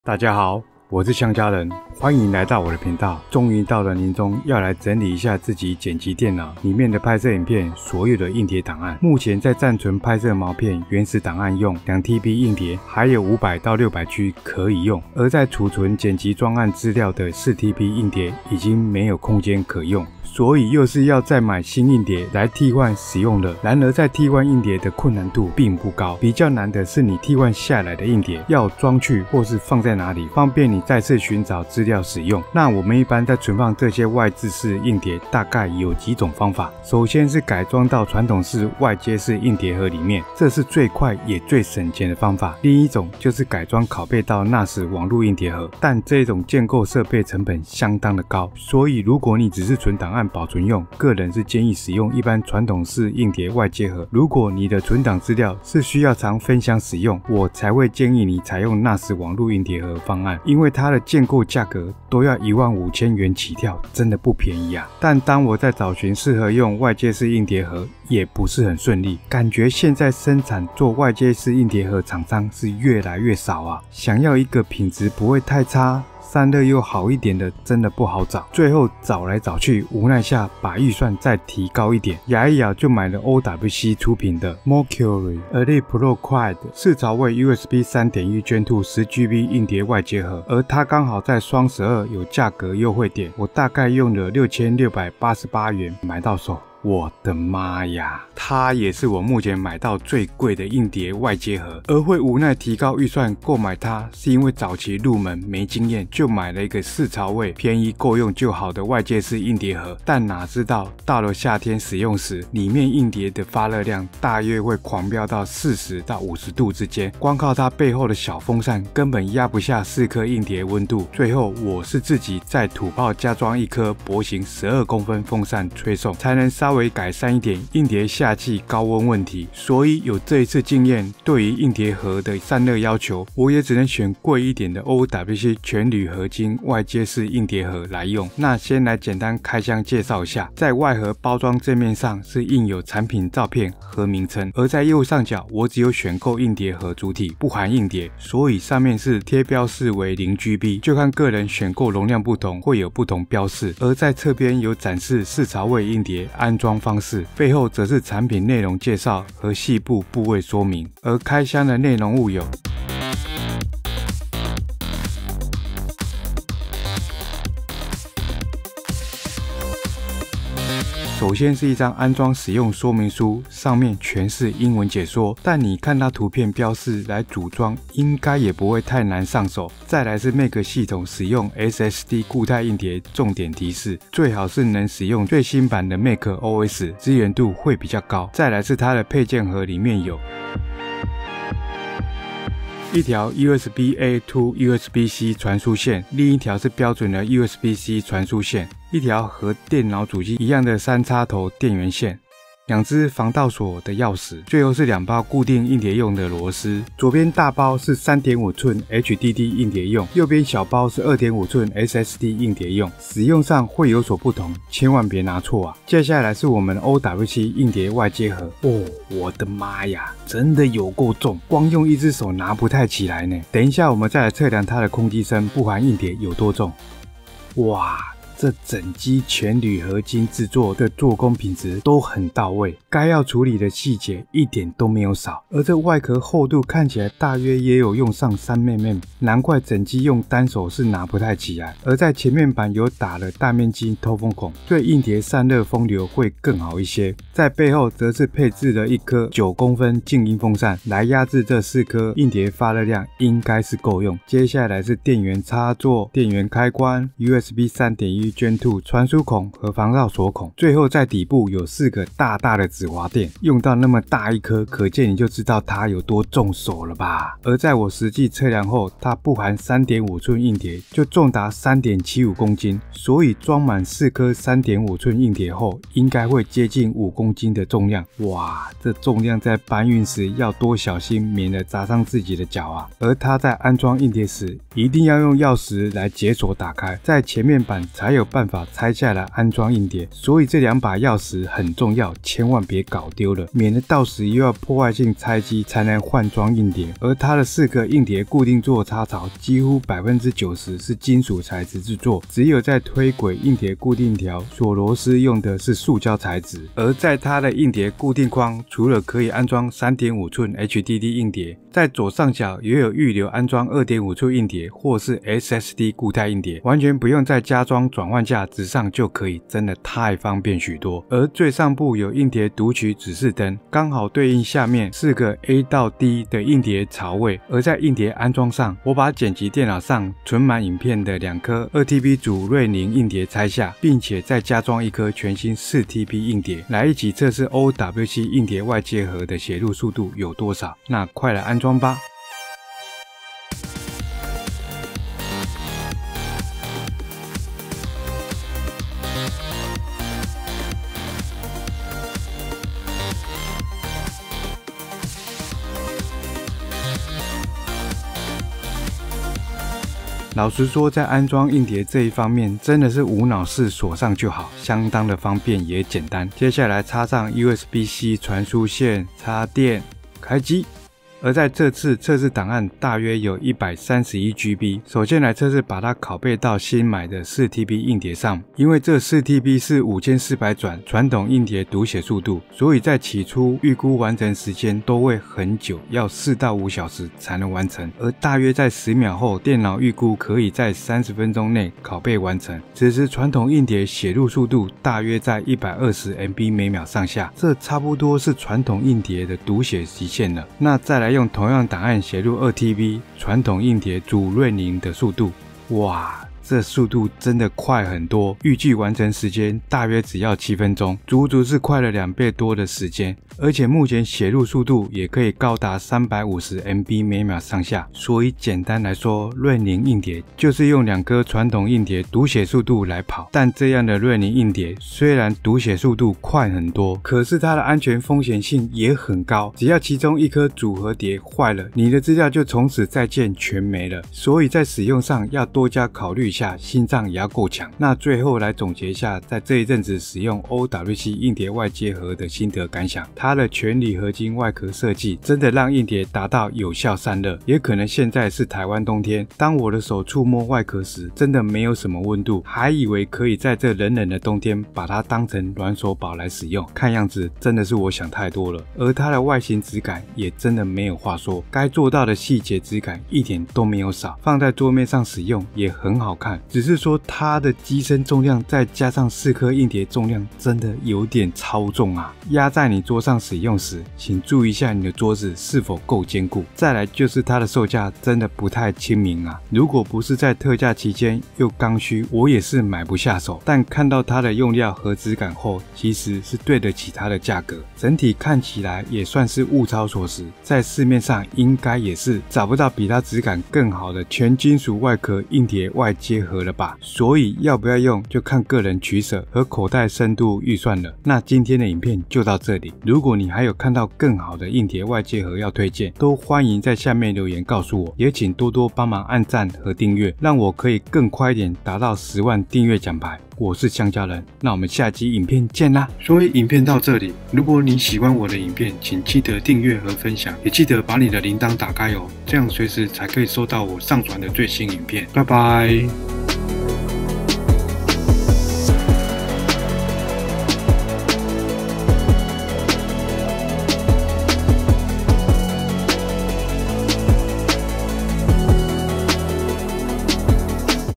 【香教仁】做視頻人的悲哀啊！永遠不夠用和買不完的硬碟！｜全屬材質OWC Mercury Elite Pro Quad硬碟外接盒實測『CC字幕』
終於到了年終要來整理一下自己剪輯電腦。
裡面的拍攝影片所有的硬碟檔案，
目前在暫存拍攝毛片原始檔用2TB硬碟還有5～600GB可以用。
而在儲存剪輯專案資料的4TB硬碟已經沒空間可用，
所以又是要再買新硬碟來替換使用了！